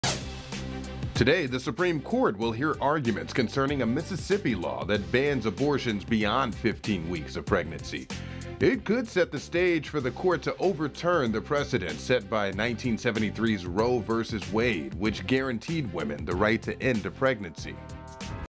Abortion rights at stake in historic Supreme Court arguments Wednesday
Abortion rights are on the line at the Supreme Court in historic arguments over the landmark ruling nearly 50 years ago that declared a nationwide right to end a pregnancy. The justices on Wednesday will weigh whether to uphold a Mississippi law that bans abortion after 15 weeks and overrule the 1973 Roe v. Wade decision.

Mississippi also is asking the court to overrule the 1992 ruling in Planned Parenthood v. Casey, which reaffirmed Roe. The arguments can be heard live on the court’s website, starting at 10 a.m. EST.

The case comes to a court with a 6-3 conservative majority that has been transformed by three appointees of President Donald Trump, who had pledged to appoint justices he said would oppose abortion rights.